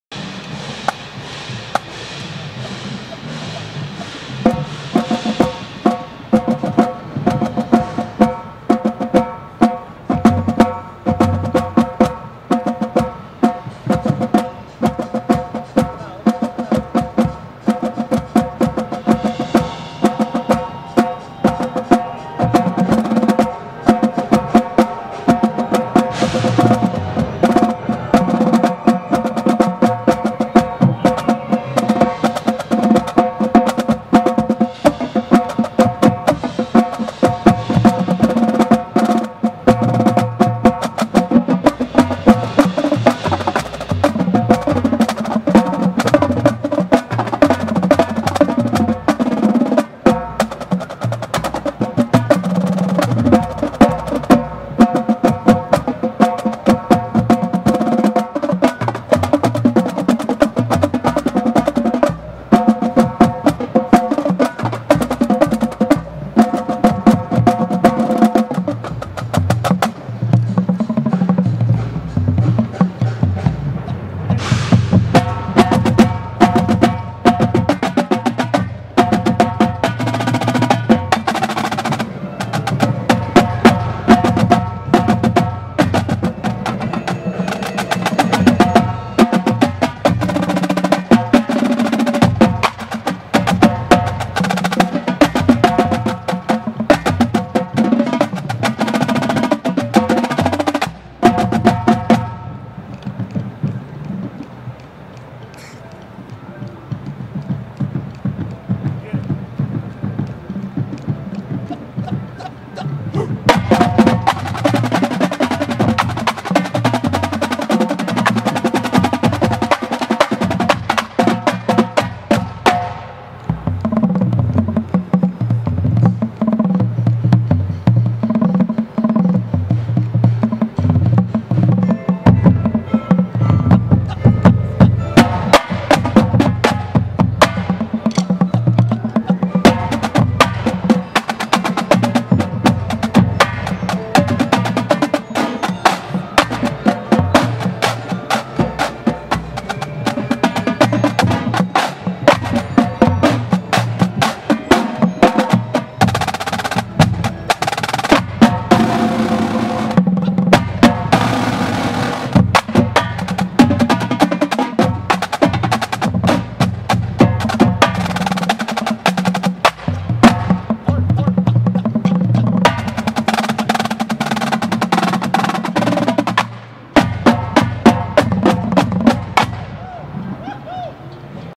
2009 Blue Devils Opener DCI Denver HD
2009 Blue Devils drumline playing through their show music in the parking lot in Denver Colorado.